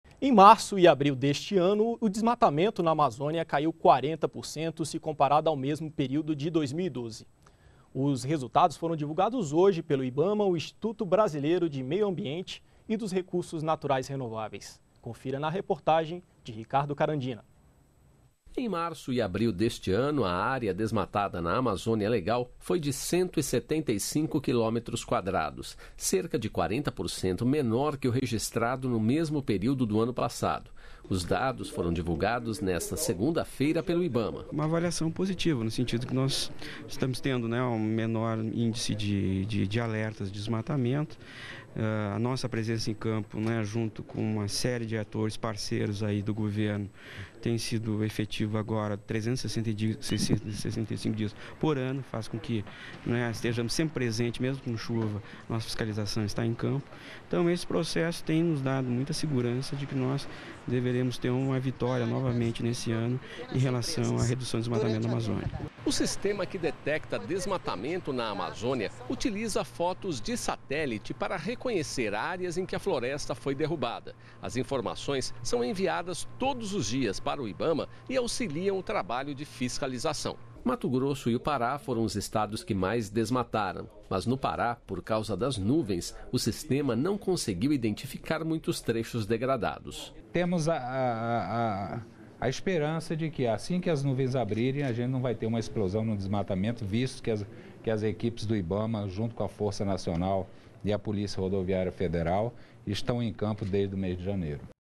Diminui o desmatamento na Amazônia nos meses de março e abril
NBR NOTÍCIAS -- 06.05.13: Em março e abril deste ano, o desmatamento na Amazônia caiu 40% se comparado ao mesmo período de 2012. O resultado foi divulgado pelo Instituto Brasileiro do Meio Ambiente e dos Recursos Naturais Renováveis (Ibama). Na região da Amazônia Legal,  a área desmatada foi de 175 km². Considerado positivo, o monitoramento é realizado  por um sistema que detecta o desmatamento na Amazônia com fotos de satélite. Os dados são enviados todos os dias para o Ibama e auxiliam o trabalho da fiscalização.